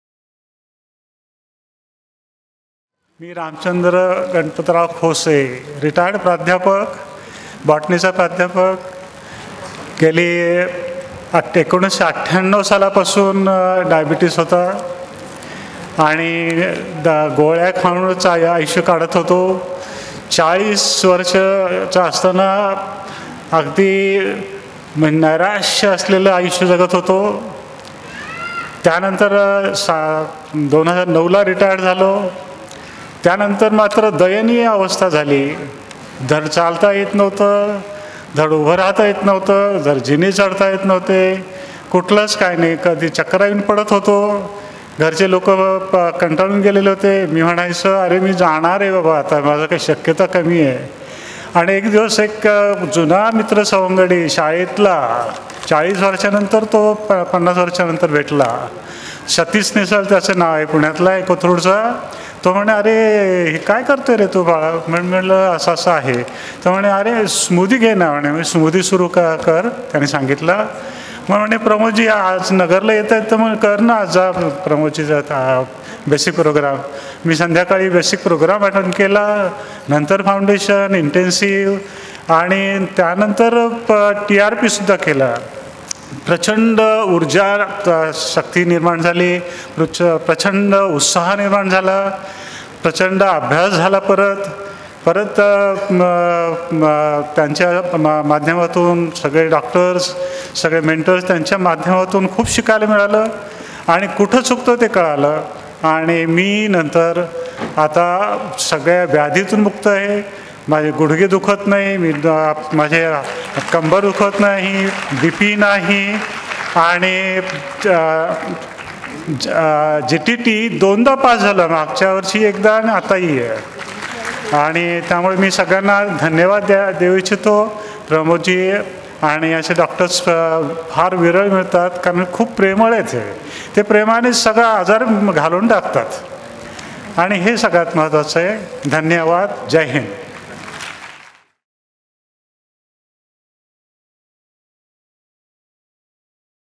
Diabetes Freedom at Muktotsav - Freedom Story by Ram Khose
At MUKTOTSAV 2017, on 12th November, Prof Ram Khose, diabetic since 1998, sharing his transformational experience by following the Freedom from Diabetes natural path. His description of becoming free of medicines, negativity, depressing feelings and many health problems is sure to touch your heart and inspire you. He has passed the GTT test TWICE and now a ROLE MODEL for many.